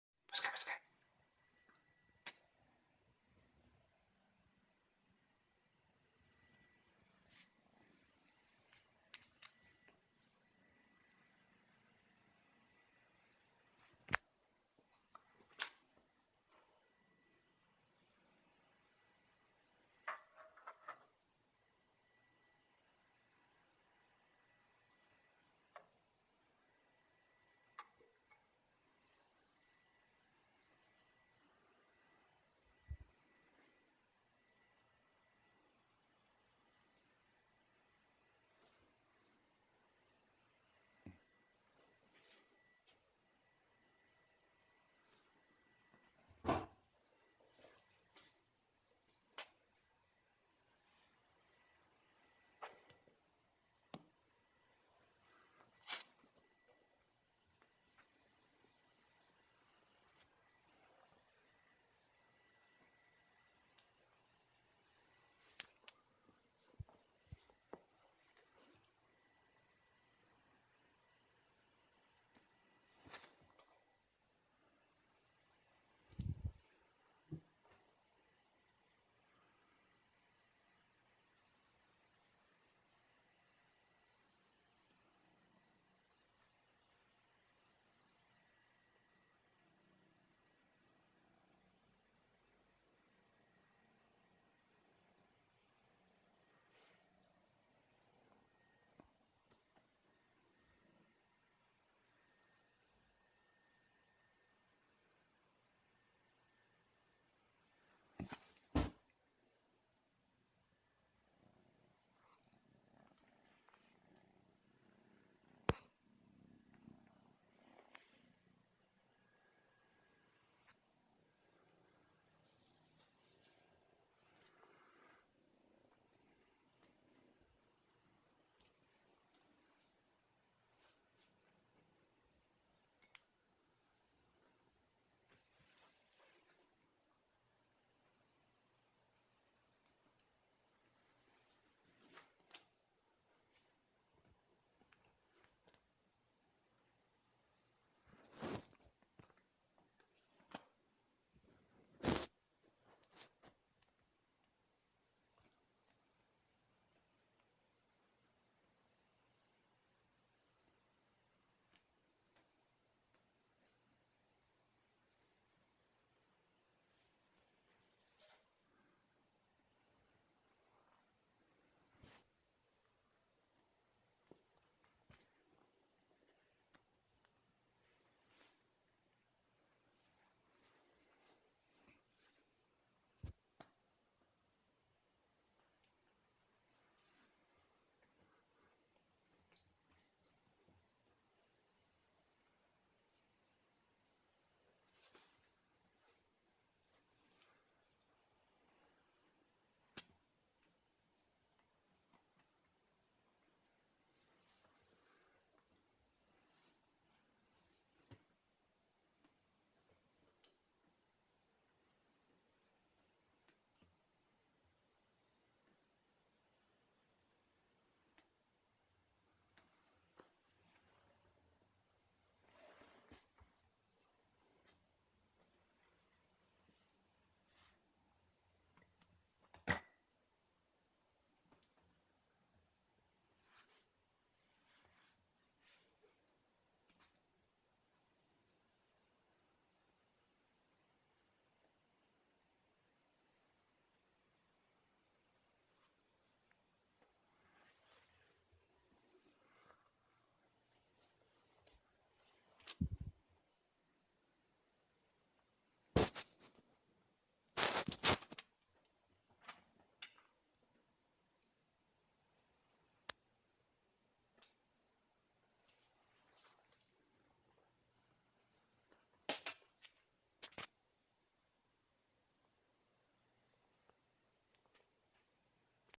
Ragdoll Kitten Playing With Mouse
13 week old Rag Doll kitten playing with a mouse.